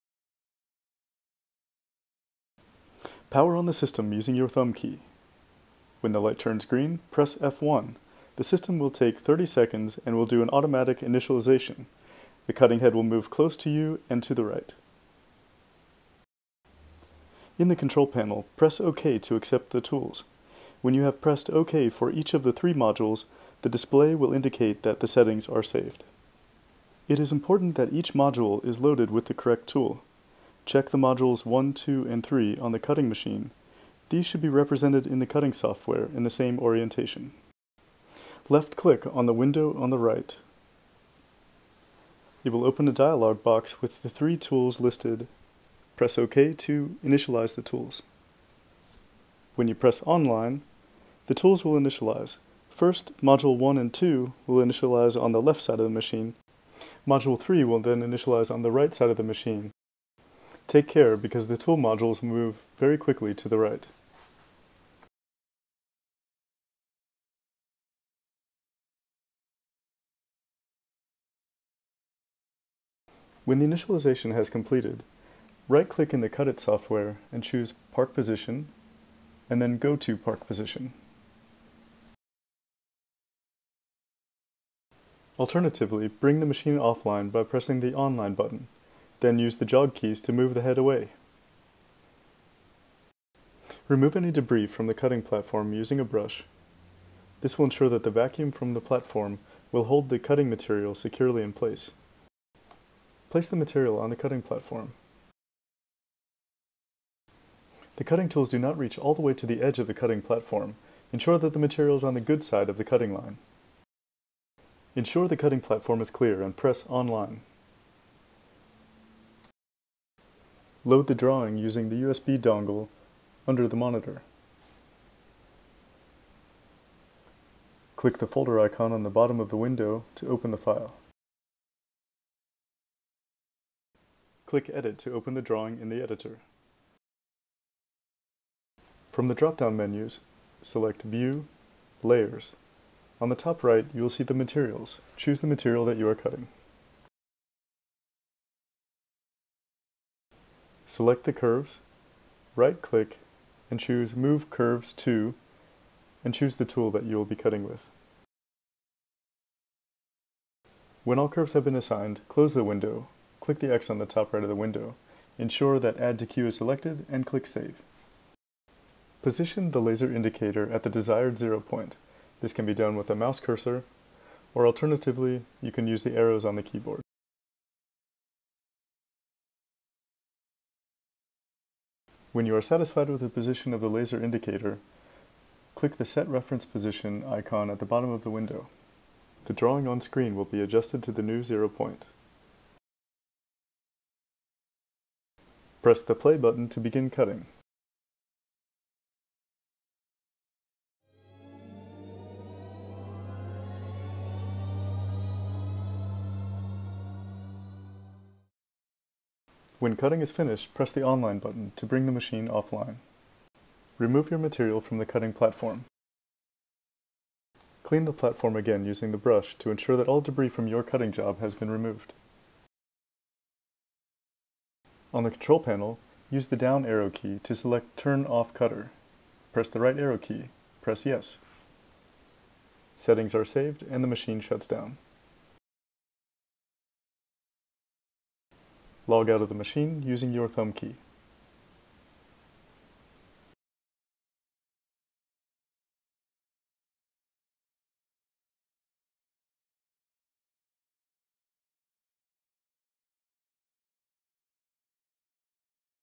CNC KnifeCutting Part 1
These are the basic steps to power on the machine, load and cut the drawing, and then power down and log off the machine.

1-Power on the system using your thumb key
2-Press F1 on the control panel, wait 30 seconds
3-Press OK for each tool to confirm
4-Calibrate tools
5-Move the cutting tools to the park position
6-Clean the cutting table with the brush
7-Place material on cutting table above cutting line
8-Load your drawing
9-Assign the curves to the desired tool
10-Close edit window and send drawing to queue
11-Move the laser indicator to the desired “0” axis and click the “set 12-reference position” icon
13-Press the “Play” button to begin cutting
14-Press “Online” to bring the machine offline
15-Remove cutting materials and clean platform
16-Select “turn off cutter” from the control panel
17-Log out of the machine using your thumb key